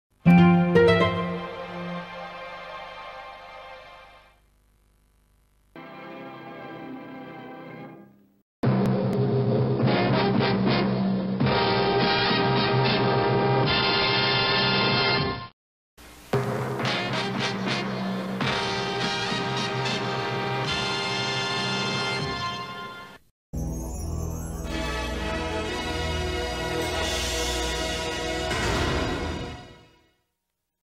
ITC Entertainment Logo History Simplified
Major British television distributor. Was folded to Universal and assets were sold into Carlton. Founded by Lord Grade and based in London.
0:00 Signation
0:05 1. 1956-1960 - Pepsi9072
0:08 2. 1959-May 21, 1975 - news5aksionfan661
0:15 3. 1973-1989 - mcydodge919
0:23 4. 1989-October 11, 1998 - Pepsi9072